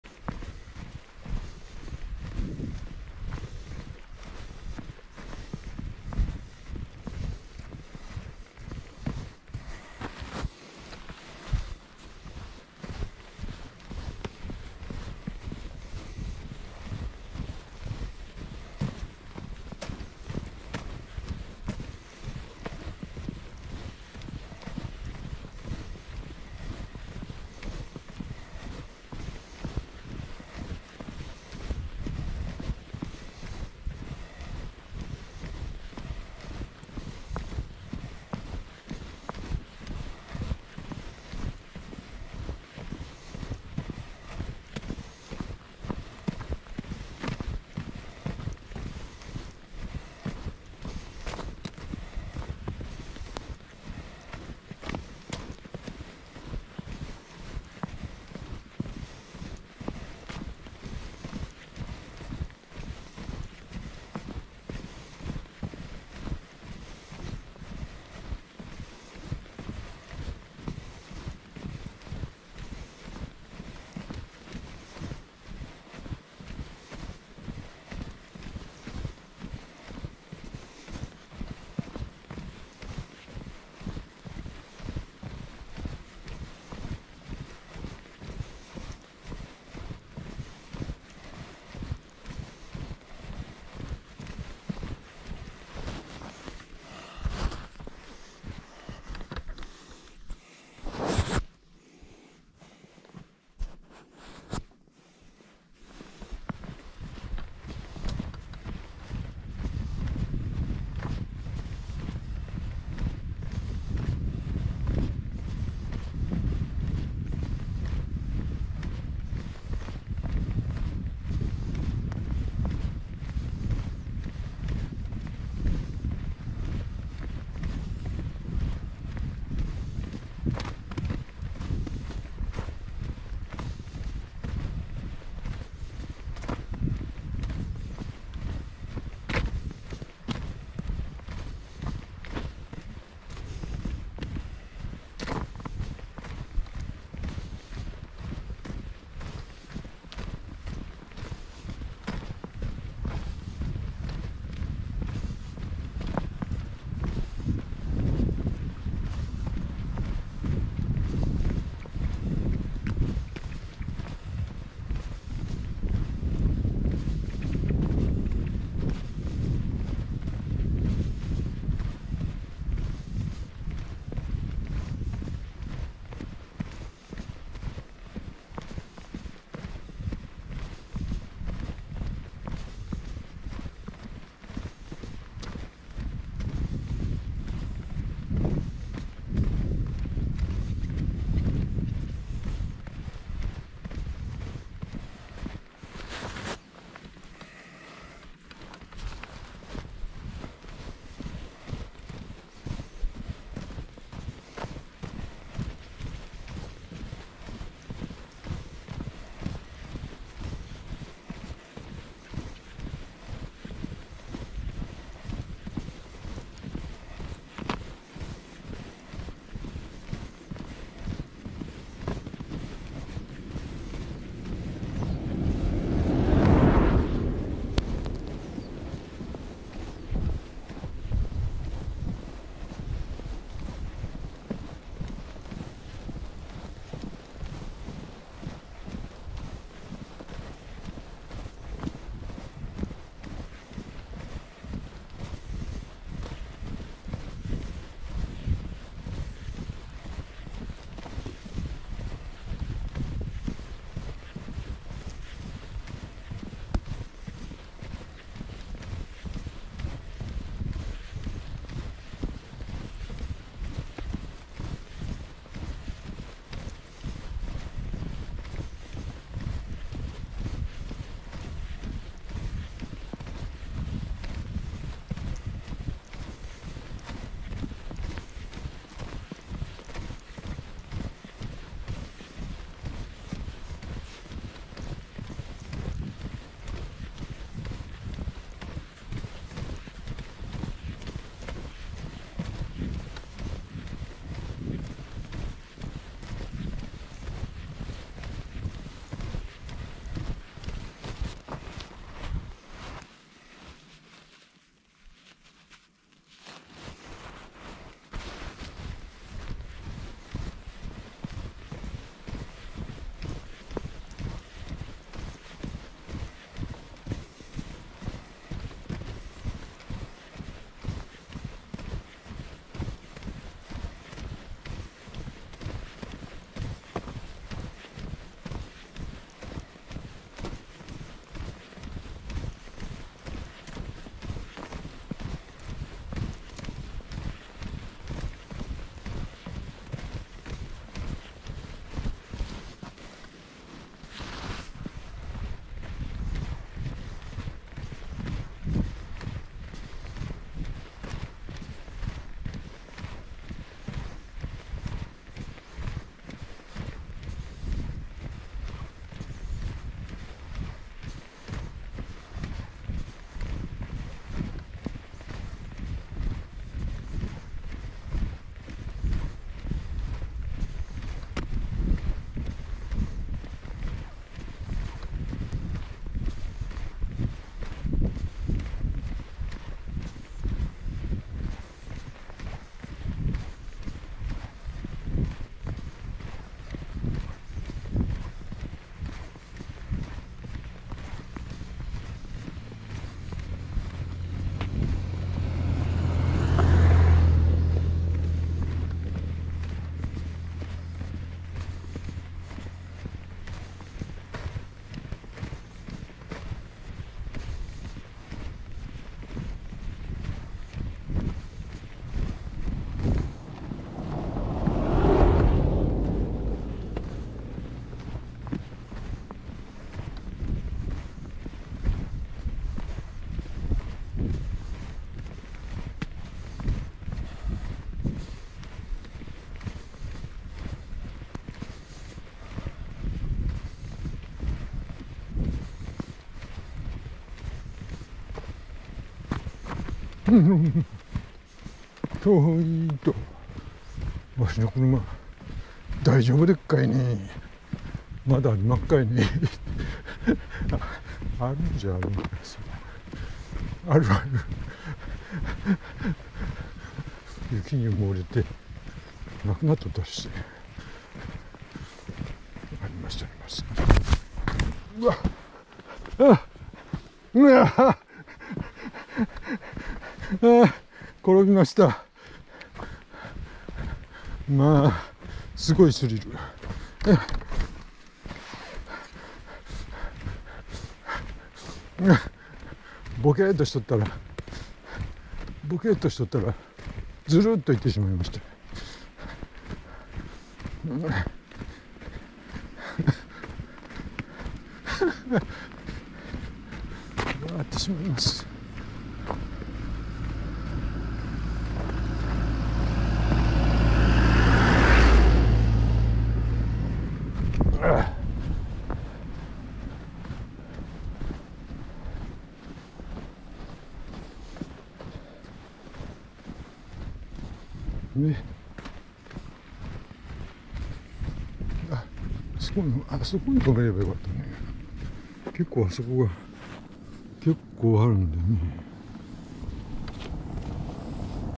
滑った！ 元日 正月 1月1日 金曜 大雪 鳥取県道36号名和岸本線を大山ガーデンプレイスに向かいます ガーデンプレイス手前で滑って転びました 鳥取県西伯郡伯耆町丸山 大山ガーデンプレイス
今日の散歩道フォトライブラリー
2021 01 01 ガーデンプレイスから❄
滑った！
 元日 正月 1月1日 金曜 大雪
 鳥取県道36号名和岸本線を大山ガーデンプレイスに向かいます
 ガーデンプレイス手前で滑って転びました
 鳥取県西伯郡伯耆町丸山 大山ガーデンプレイス